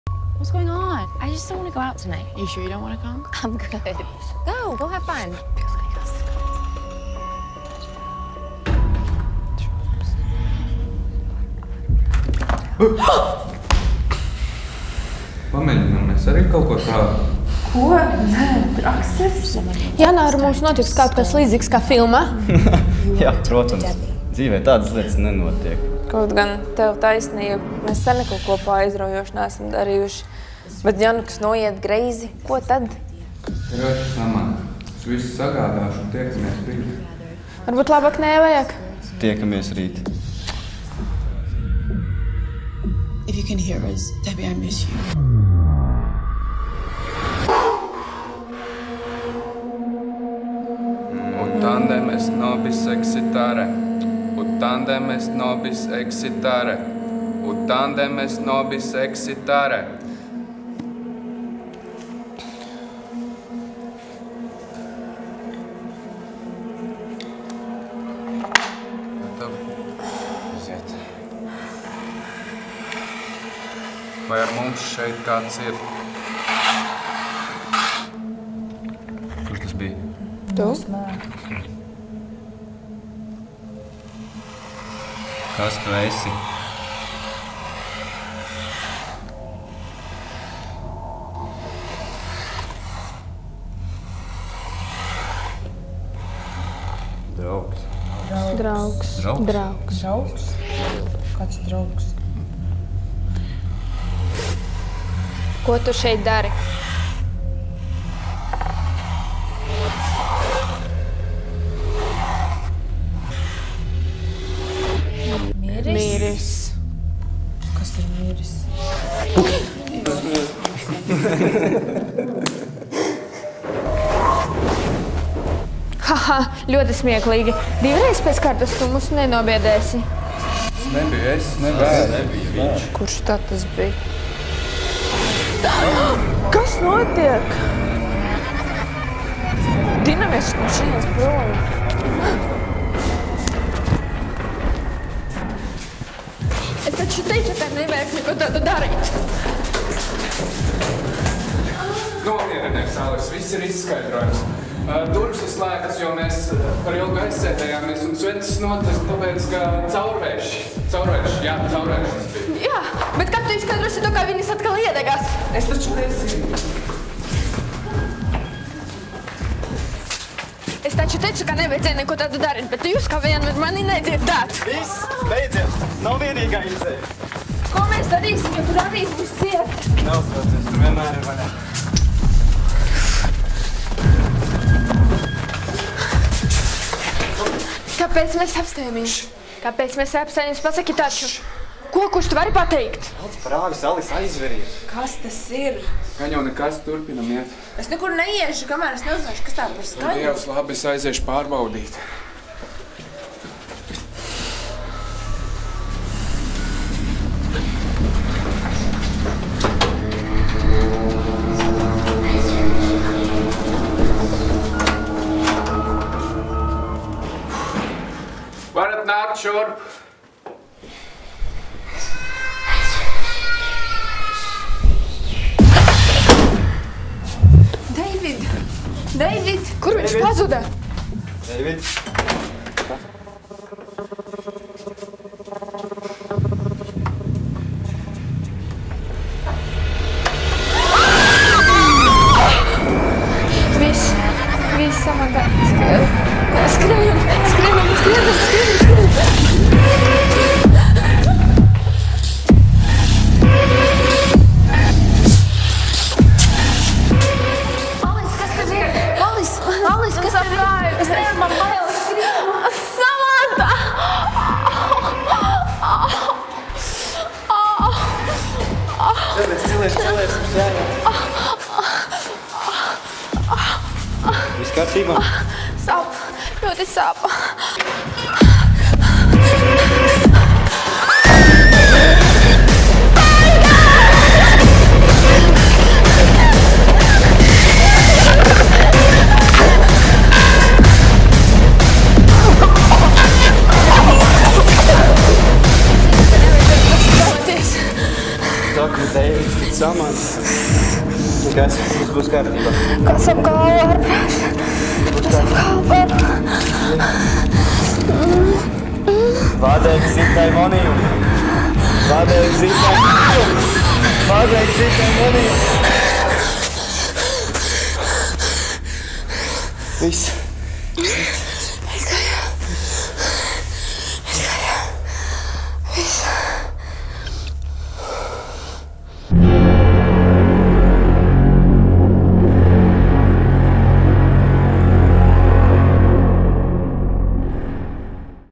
ŠAUSMENE
Šaušalīgs stāsts par šaušalīgu gadījumu, kas var gadīties ikvienam, ja tas izklaidējas ar šaušalīgām spēlēm šaušalīgos apstākļos.
BRĪDINĀJUMS - Skatītājus ar vājiem nerviem un bērnus - lūgums turēt tālāk no ekrāniem!
Autors Ivo Buģis.